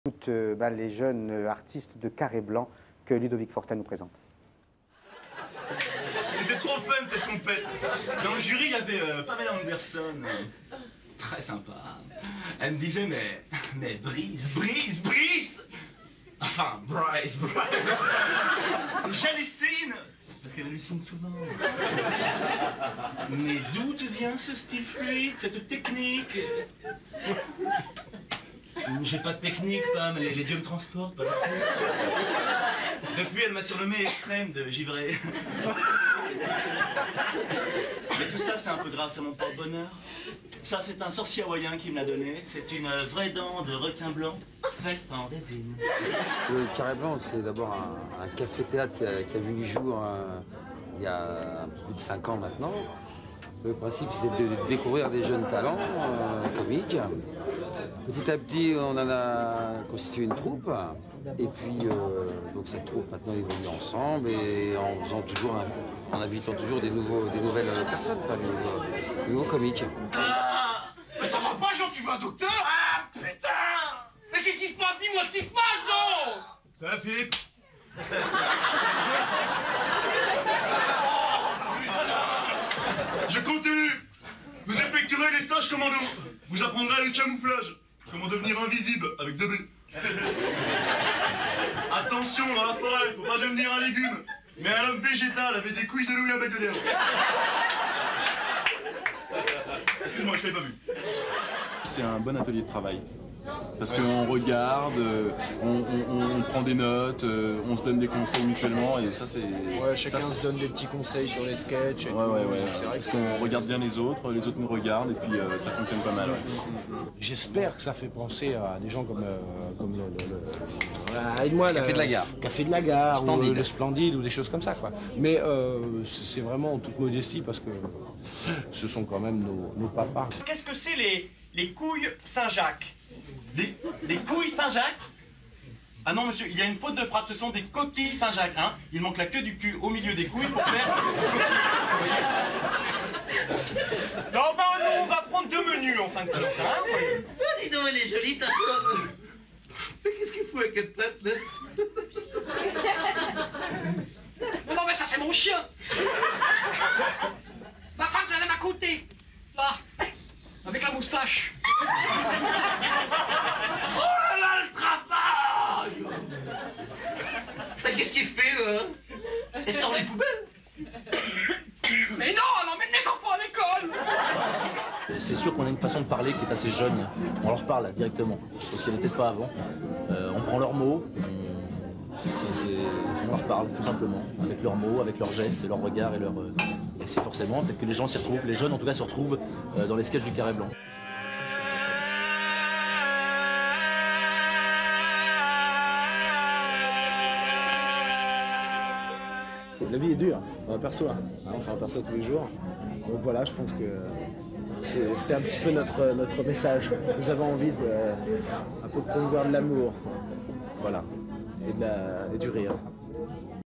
Jean Dujardin en 1997 😱// Extrait archives M6 Media Bank
En 1997, Jean Dujardin montait sur scène au Carré Blanc à Paris pour jouer ses sketchs, bien avant qu’il ne devienne la star que l’on connaît aujourd’hui ! 🎤